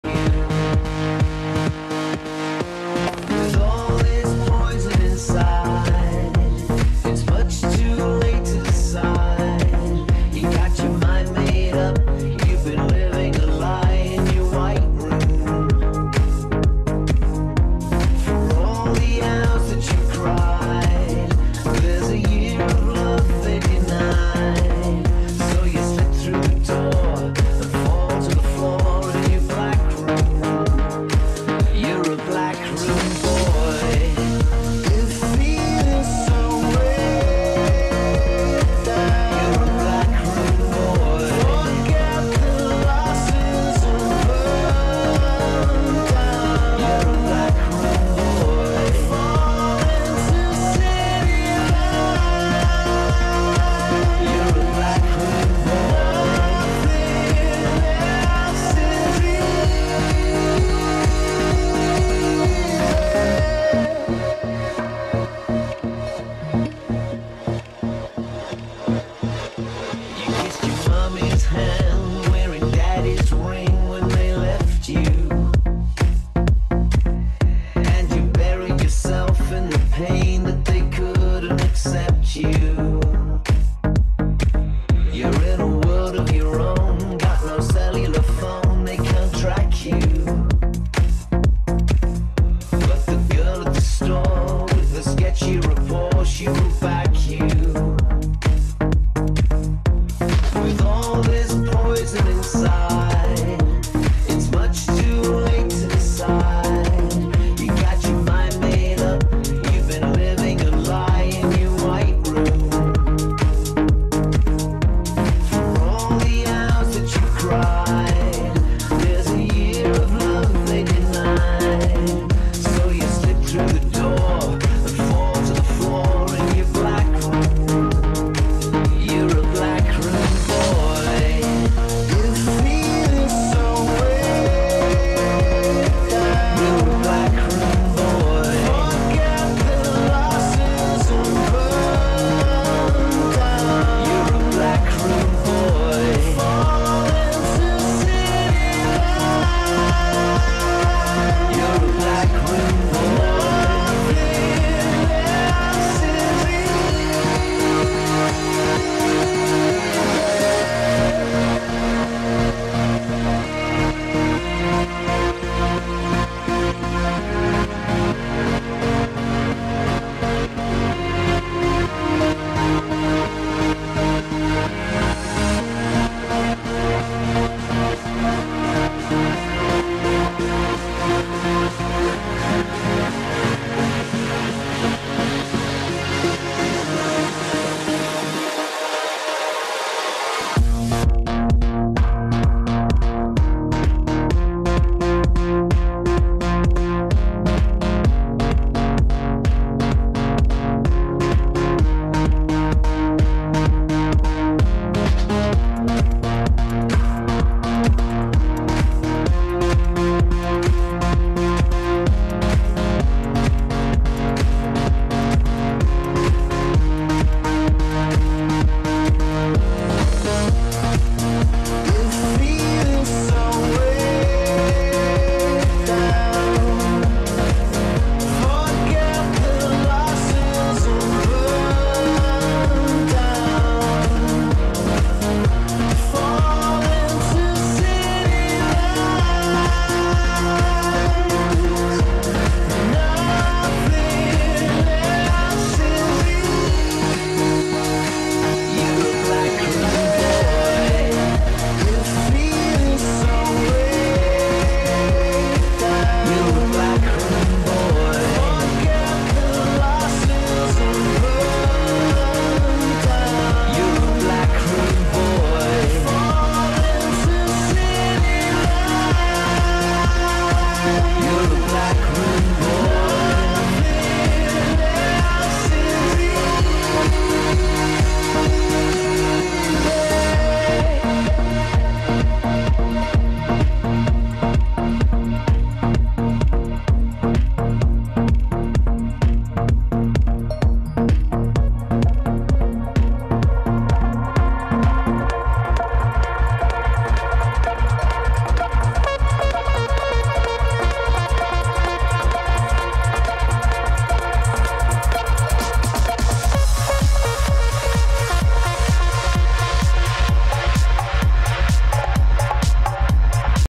"Epic EDM Party Mix 🔥 | DJ & Dancer Take You on a Night to Remember!"
🎶✨ Get ready to vibe with our special electronic music mix! This curated set is packed with electrifying beats and unforgettable melodies that will elevate your mood and keep you dancing. 💃🕺
Want to experience the full mix? Head over to GoldSuga.tv for the complete journey!